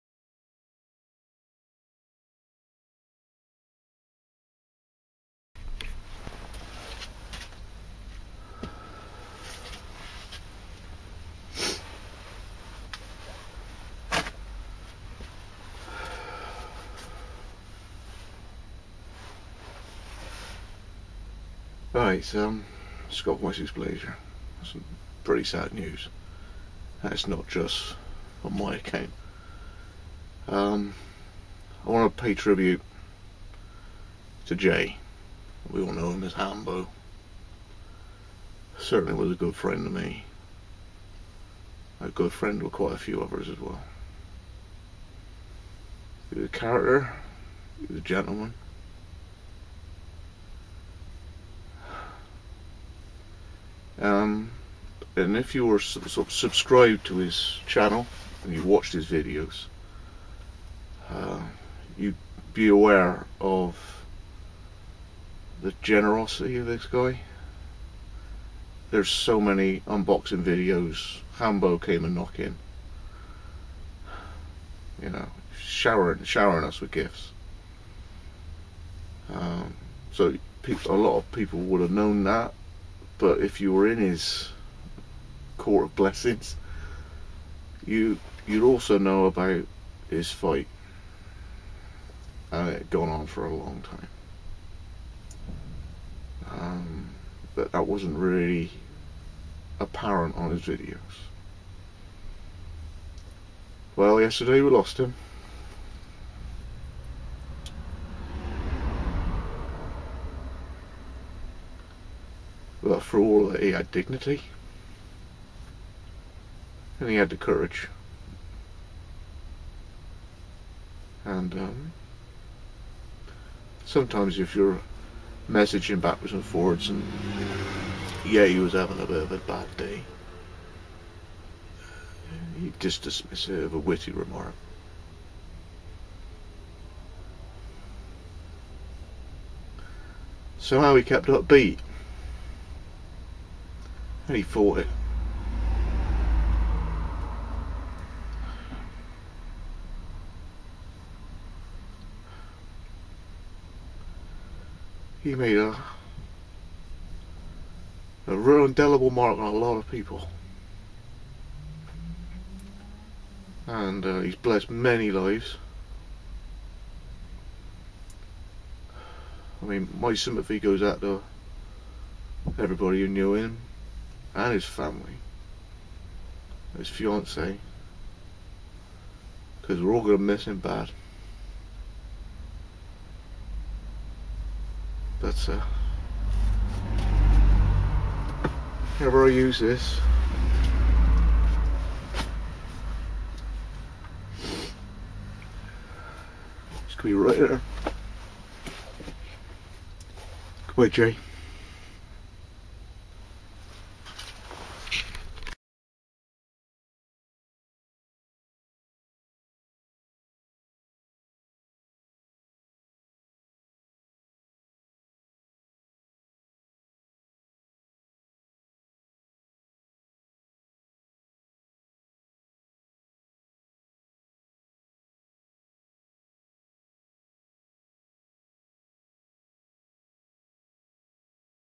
Hambeau, A Tribute to a Friend
A light faded away yesterday and we lost a treasured brother with it, a generous and jovial knife enthusiast who was always there with a humourous tone and a courageous heart. Farewell Hambeau, we won't forget you.